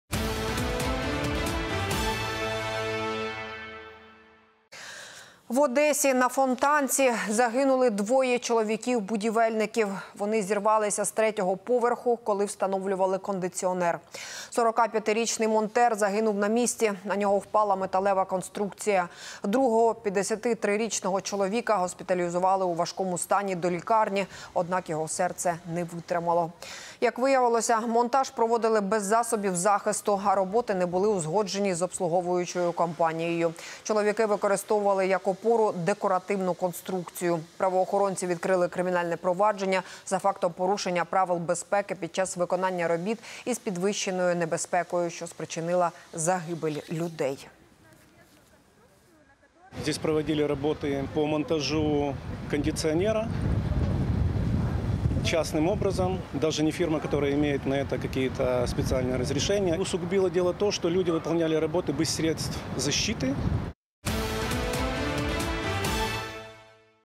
В Одесі загинуло двоє чоловіків-будівельників: на одного впала металева конструкція
У селищі Фонтанка Одеської області із третього поверху зірвались двоє чоловіків. Вони встановлювали у будівлі кондиціонер.

Один загинув на місці – на нього впала металева конструкція і придавила. Іншого госпіталізували у важкому стані до лікарні, однак серце чоловіка не витримало.